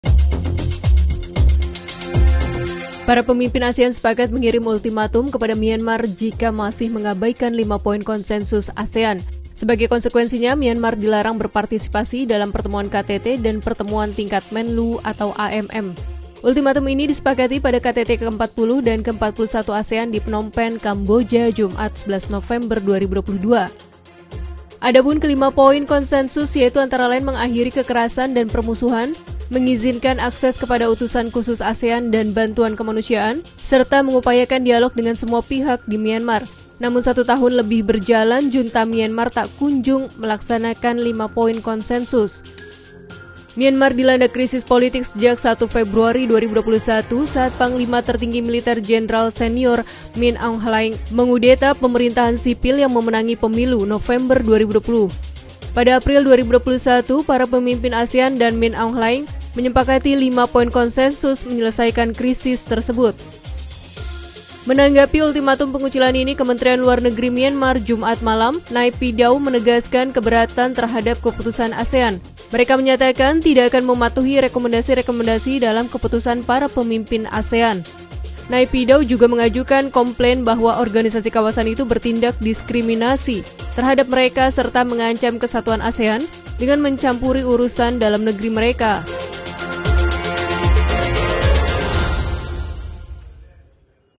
ASEAN Ultimatum Junta Militer Myanmar Jika Masih Abaikan Konsenus
Para pemimpin ASEAN sepakat mengirim ultimatum kepada Myanmar jika masih mengabaikan lima poin konsensus ASEAN. Sebagai konsekuensinya, Myanmar dilarang berpartisipasi dalam pertemuan KTT dan pertemuan tingkat menlu (AMM). Ultimatium ini disepakati pada KTT ke-40 dan ke-41 ASEAN di Phnom Penh, Kamboja, Jumat (11/11/2022).

Ultimatium ini disepakati pada KTT ke-40 dan ke-41 ASEAN di Phnom Penh, Kamboja, Jumat (11/11/2022).

Adapun Kelima poin konsensus yaitu antara lain mengakhiri kekerasan dan permusuhan, mengizinkan akses kepada utusan khusus ASEAN dan bantuan kemanusiaan, serta mengupayakan dialog dengan semua pihak di Myanmar. Namun, satu tahun lebih berjalan, junta Myanmar tak kunjung melaksanakan lima poin konsensus.

Myanmar dilanda krisis politik sejak 1 Februari 2021, saat Panglima Tertinggi Militer Jenderal Senior Min Aung Hlaing mengudeta pemerintahan sipil yang memenangi pemilu, November 2020. Pada April 2021, para pemimpin ASEAN dan Min Aung Hlaing menyepakati lima poin konsensus menyelesaikan krisis tersebut.

Menanggapi ultimatum pengucilan ini, Kementerian Luar Negeri Myanmar, Jumat malam, Naypyidaw menegaskan keberatan terhadap keputusan ASEAN. Mereka menyatakan tidak akan mematuhi rekomendasi-rekomendasi dalam keputusan para pemimpin ASEAN.
Naypyidaw juga mengajukan komplain bahwa organisasi kawasan itu bertindak diskriminasi terhadap mereka serta mengancam kesatuan ASEAN dengan mencampuri urusan dalam negeri mereka.